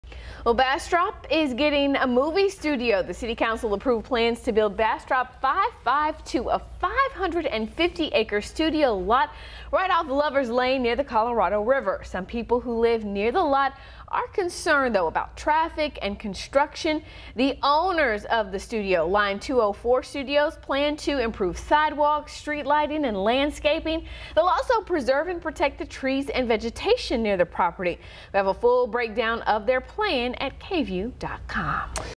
Bastrop City Council approves 550-acre film studio development | KVUE
The council unanimously voted to give the green light to an amended plan for the "Bastrop 552" development at its meeting on Tuesday.